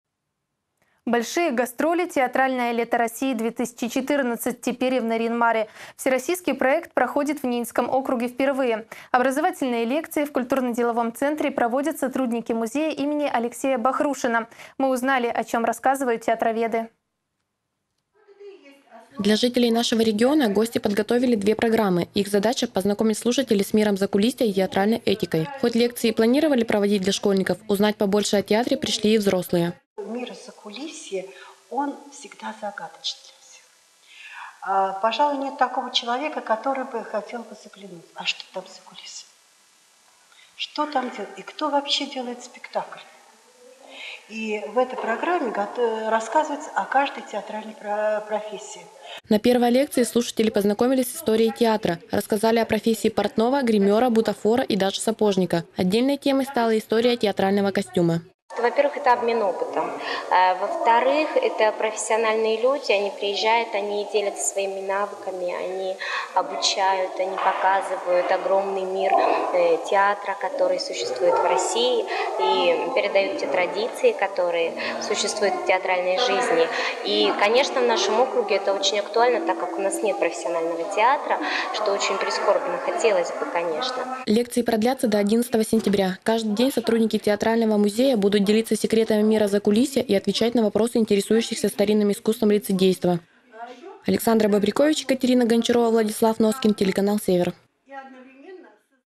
В Нарьян-Мар приехали «Большие гастроли»
«Большие гастроли. Театральное лето России — 2014» теперь и в Нарьян-Маре! Всероссийский проект проходит в Ненецком округе впервые. Образовательные лекции в культурно-деловом центре проводят сотрудники музея имени Алексея Бахрушина. Мы побывали на первой беседе с театроведами.